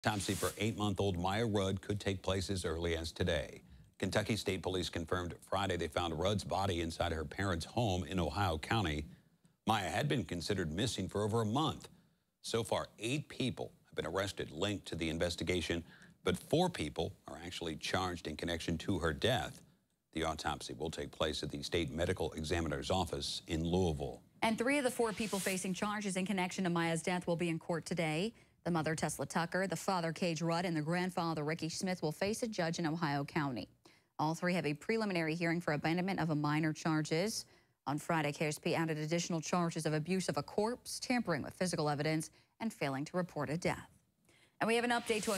Family of deceased Ohio County infant Miya Rudd appearing in court
Family members of an Ohio County infant whose remains were discovered on Friday are appearing in court.